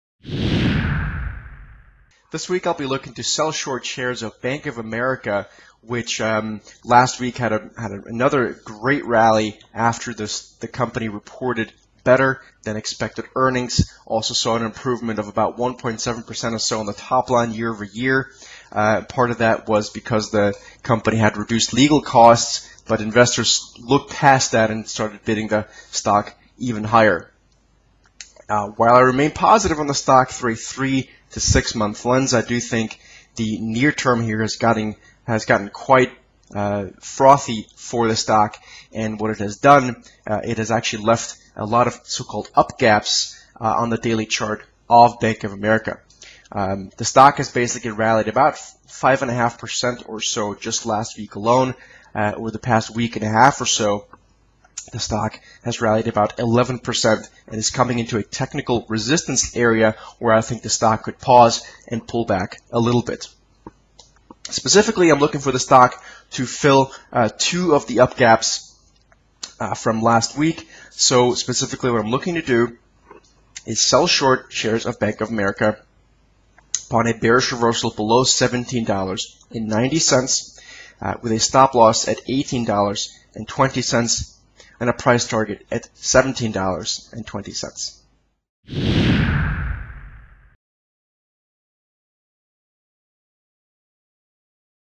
Berger: Short selling Bank of America after earnings rally
Serge Berger is looking to sell short Bank of America stock.
Better-than-expected earnings have seen Bank of America’s share price
rise 11% in less than two weeks and according to Berger it is now
hitting an area of technical resistance.
Bank of America reported its biggest quarterly profit in nearly four
years on July 15 partly due to a reduction in legal costs. Earnings
more than doubled to around USD 5 billion, while revenues rose 1.7% to
about 22 billion.
While positive on the stock in the next 3-6 months, Berger believes
the it could take a pause from the current rally and pull back. He is
looking to sell short on a bearish reversal below USD 17.90 with a
stop at 18.20 and target at 17.20.